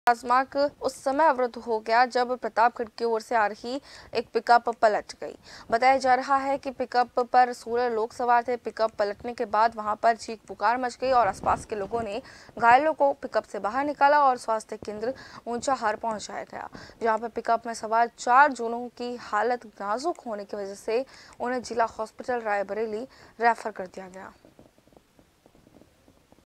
Raebareli News | राष्ट्रीय राजमार्ग पर पलटी पिकअप, 4 लोग गंभीर घायल | JAN TV
News Credit-KM